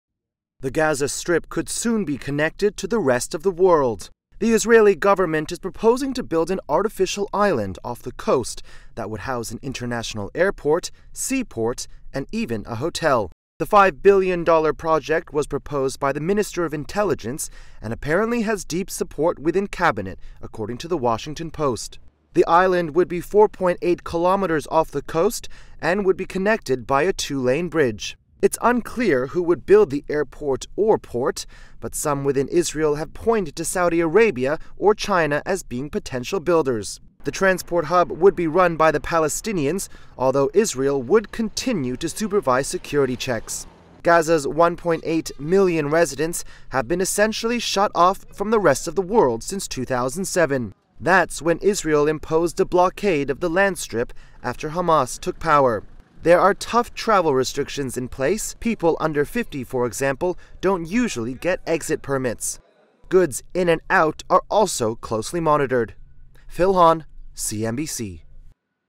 Israel to build island off Gaza strip?
A proposal by the Israeli minister of intelligence wants to see an artificial island built off the coast of the Gaza Strip. It would house an international airport, seaport and even a hotel. CNBC's Phil Han reports.

CNBC_international